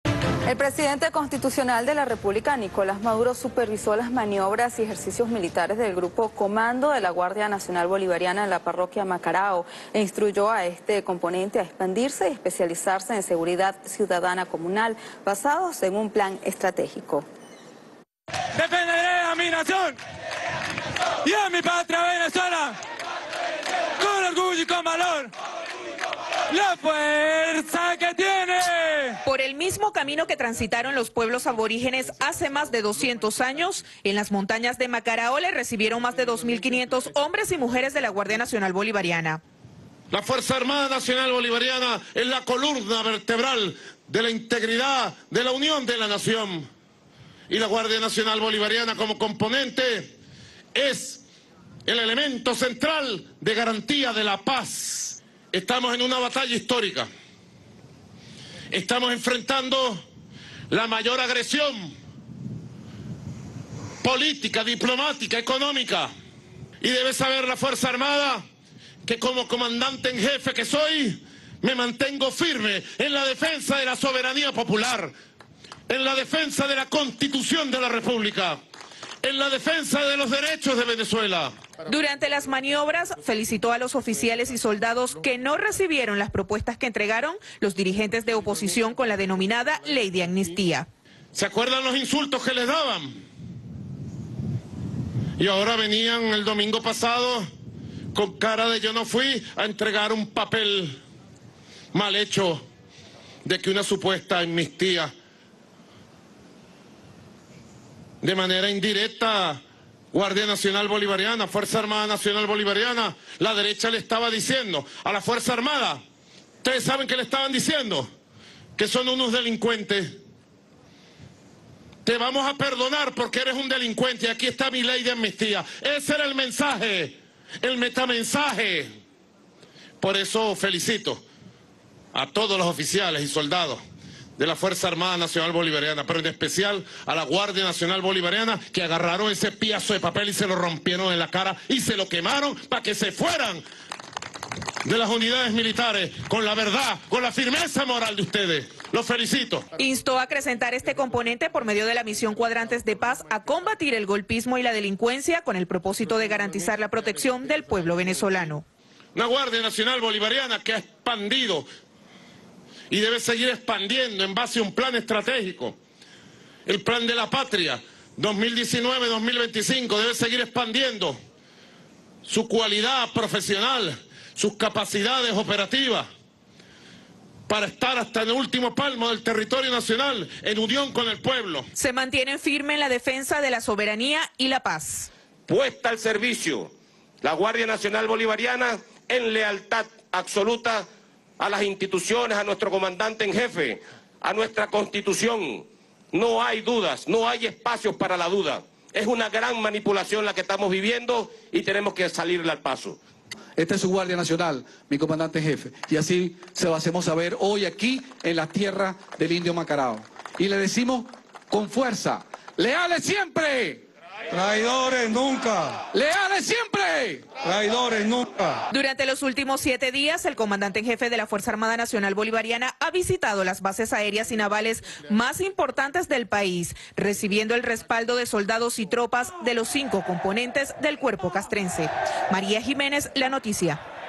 Así Maduro opinó sobre soldados que rompieron Ley de Amnistía este 1 febrero 2019
El Presidente de Venezuela, Nicolás Maduro, supervisó este viernes 1 de febrero de 2019 las prácticas militares de la Guardia Nacional Bolivariana en Macarao, Caracas, y aplaudió a los oficiales de la GNB que la semana pasada rompieron y quemaron la Ley de Amnistía, que supuestamente les garantiza "perdón" si se pliegan al golpe de Estado contra Maduro.